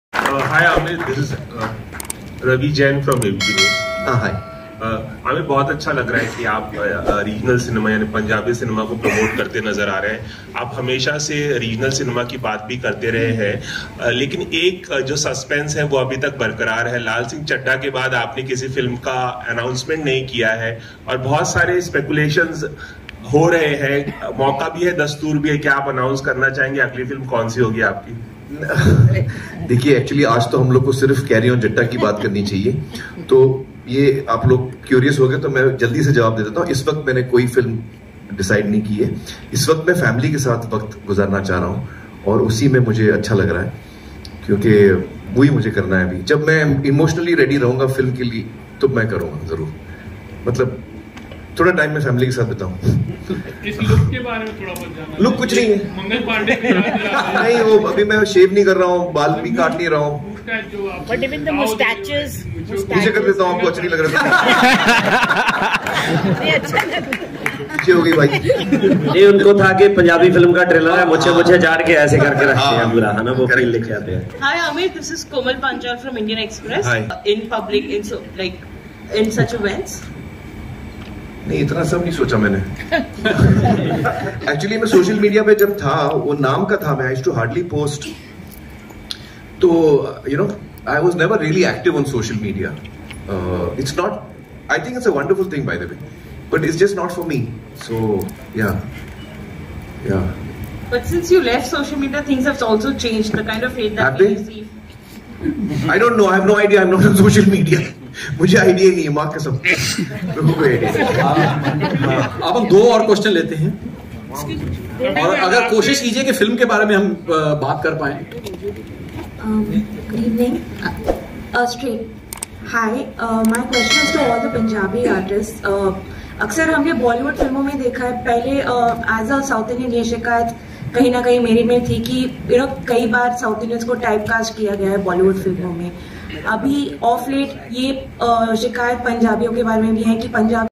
Lal Singh Chaddha के बाद कौनसी Film आमिर करने वाले है? | Must Watch Amir Khan's Reactions on this
👉Do Follow Us Now :-Instagram DM to us for Brand Collab or Promotional Queries)